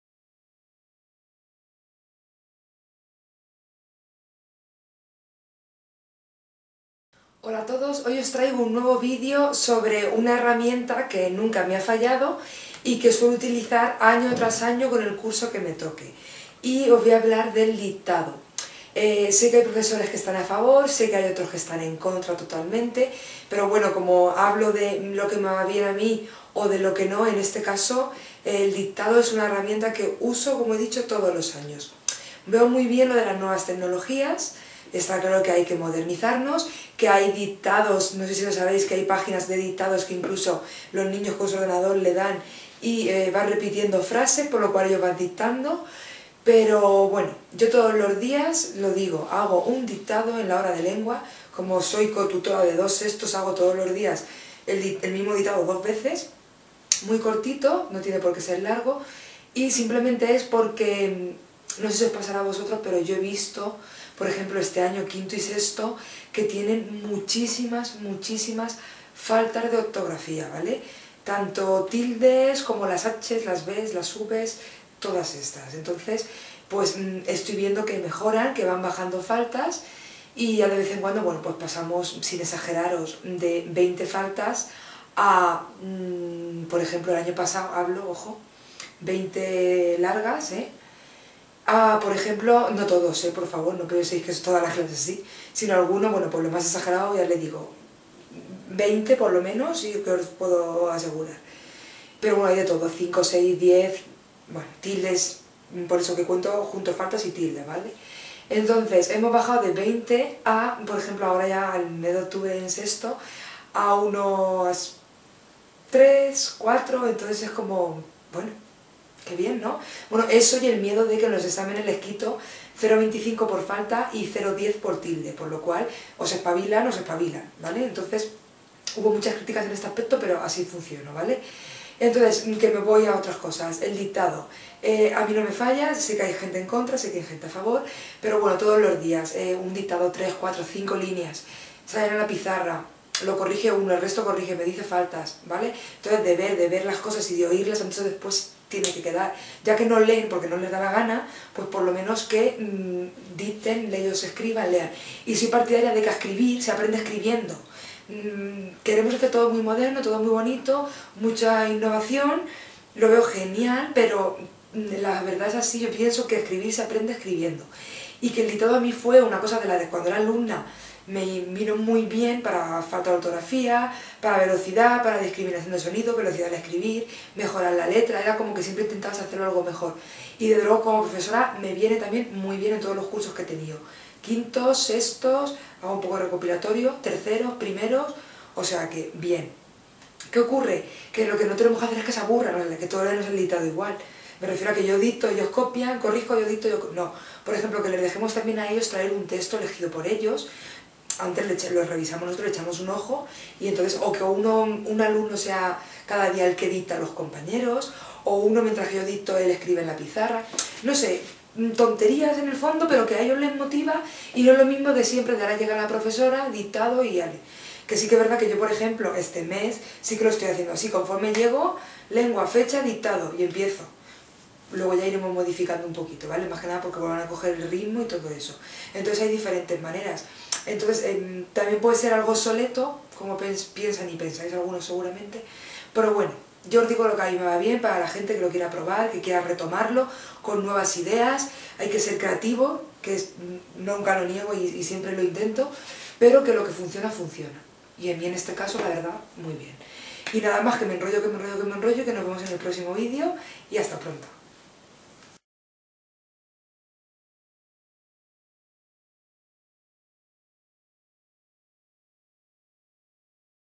El dictado en Educación Primaria. Una buena herramienta que no falla.
Hoy os traigo un vídeo sobre el dictado. Tengo pendiente otro sobre diferentes tipos de dictados, para daros alguna idea, que también podéis usar en la oposición, cuando hagáis el área de Lengua.
Seguramente muchos estaréis de acuerdo conmigo y otros muchos no tanto, pero es normal.
Veo genial las Nuevas Tecnologías, pero creo que no hay que cambiar las coas que funcionan.
A mí unas de las que me funcionan es el dictado. Creo que aprendí mucho de alumna y ahora de maestra también.
Los niños pueden ver los avances en el cuaderno, incluso podéis tener uno para solamente dictados, en los exámenes, etc.